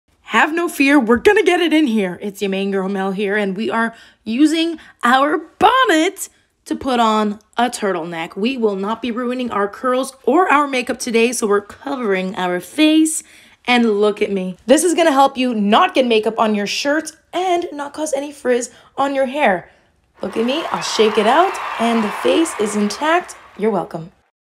LIFE SAVING Beauty Hack for protecting hair and makeup from a tight collar (or turtleneck)
If you like to do your hair and makeup before getting dressed, this trick will prevent messing up your hairstyle and creating unwanted frizz as well as avoid a makeup-stained collar

💰Rakuten How We Get Money back when we Shop Online - in order to get Cash Back you have to shop through Rakuten website -

Amazon Live